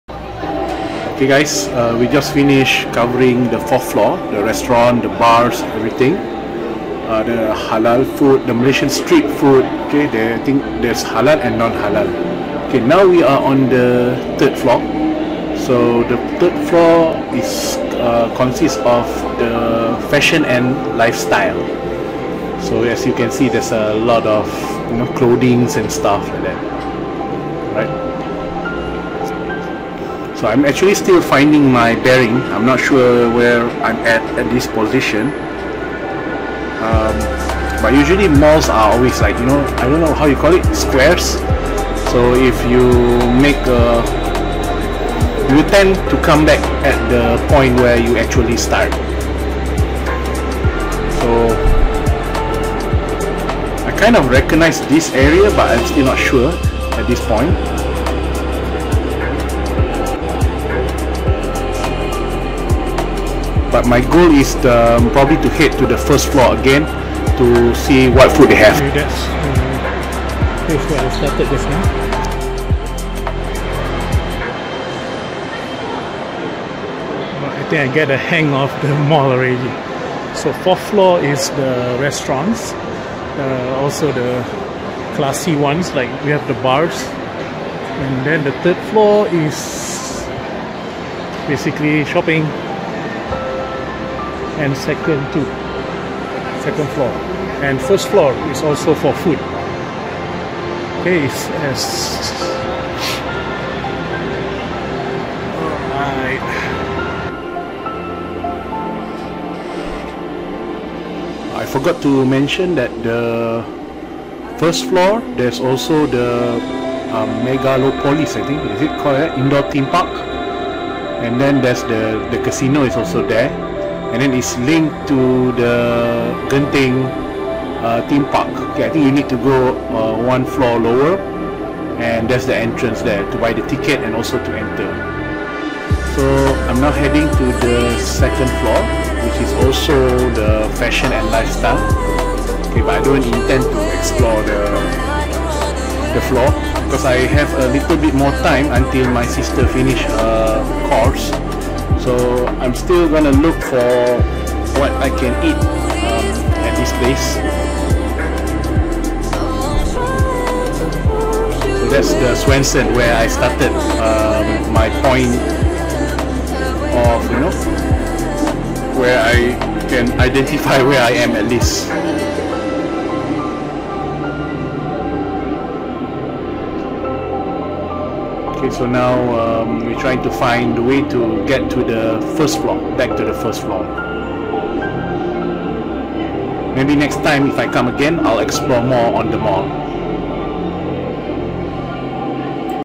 Genting Highlands & Bukit Tinggi Staycation | Day 2 | Exploring Genting Sky Avenue part 5
After i finished exploring the 4th floor (Malaysian Food Street), Restaurants and Bars, i go further down 1 floor to the 3rd floor which is fashion and lifestyle.

I'm stil searching for what i want to eat for lunch as time is getting shorter where i'm gonna meet my sister. She already has lunch at the hotel venue where she's taking the course.

So i jsut wander around then finally i saw the "swenson" and escalator that i took from ground. So i roughly now where i am now. Heading to 2nd floor and immediately to the 1st floor to have one final look on all eateries available.